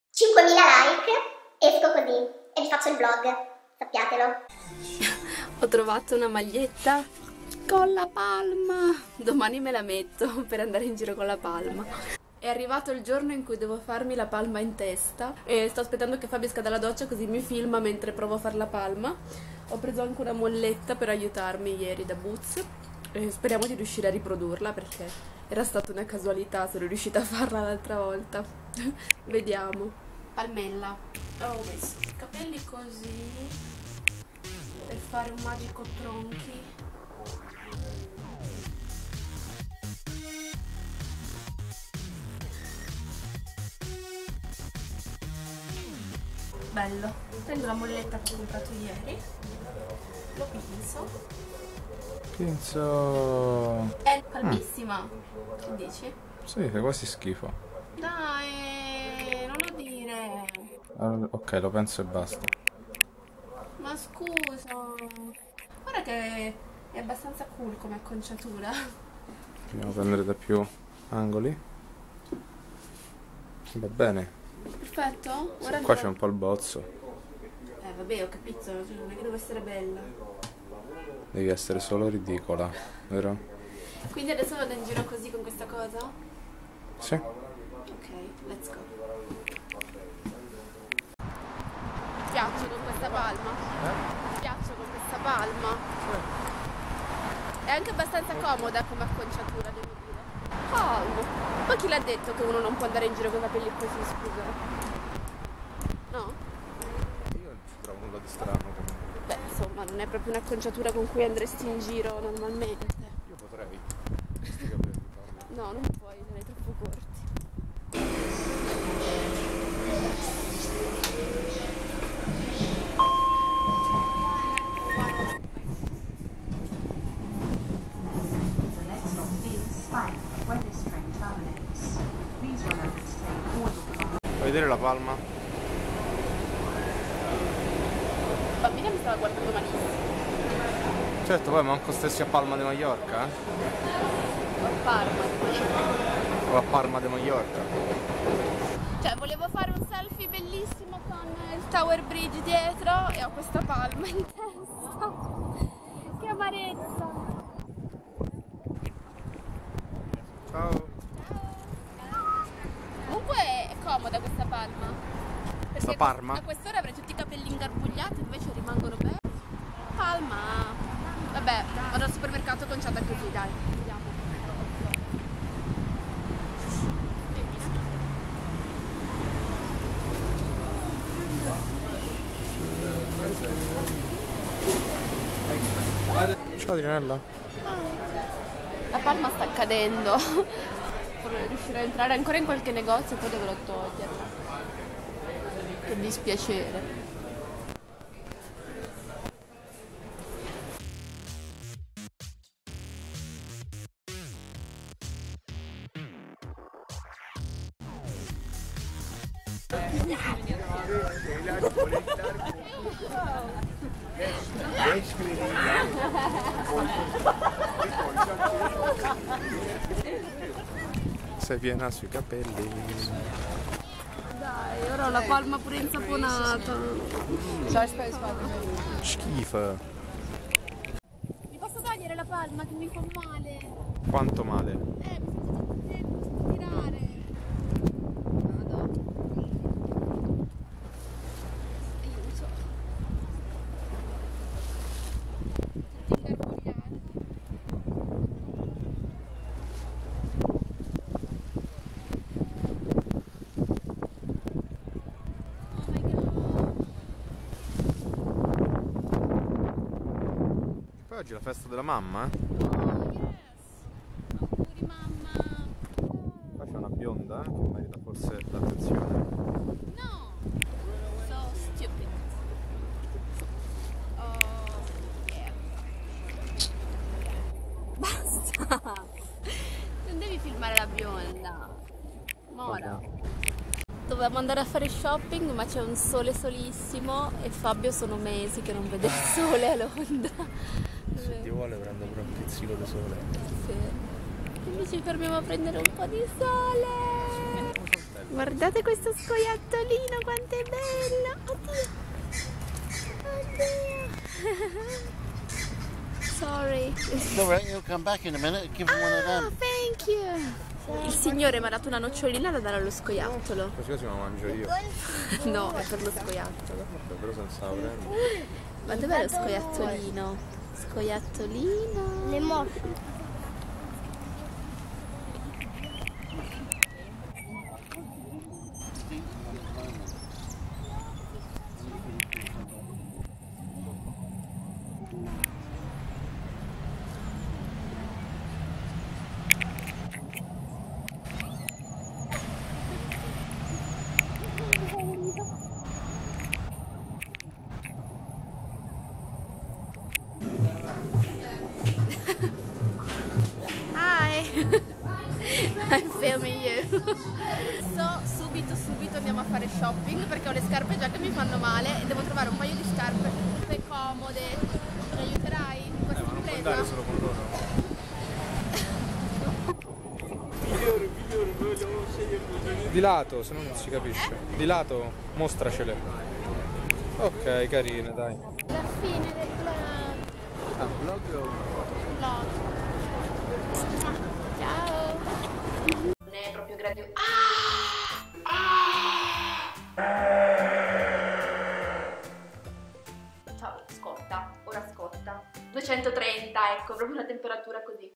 VLOG Londra con la PALMA in testa!... COME RENDERSI RIDICOLI! ;)
Come promesso 5000like raggiunti nel TESTA O CROCE TAG ed eccomi in giro per Londra con la palma in testa!!! Ahahaha Se avete altri video tag divertenti da suggerirmi commentate!

❤Makeup Tutorials - Trucchi facili e per tutte le occasioni! Anche ispirati alle celebrities come Ariana Grande, Kylie Jenner, Katy Perry ecc..